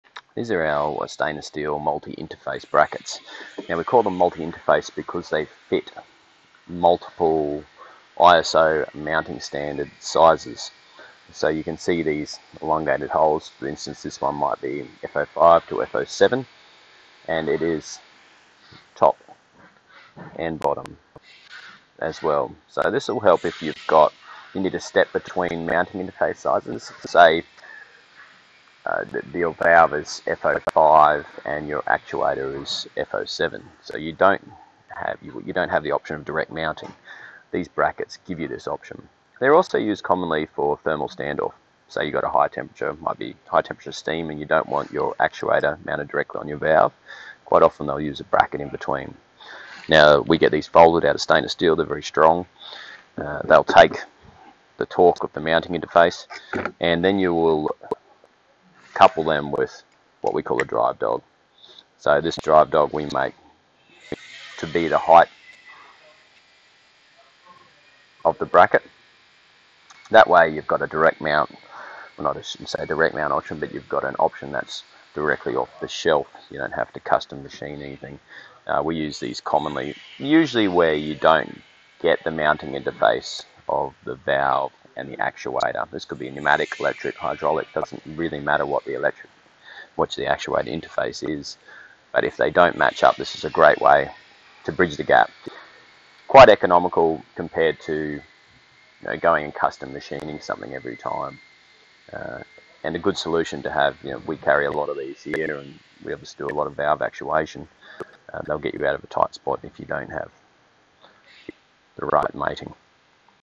Valve Mounting Brackets
Multiple interfacing Valve Brackets. This will allow mating between valves and actuators with different mounting interfaces. Also used for thermal stand off brackets.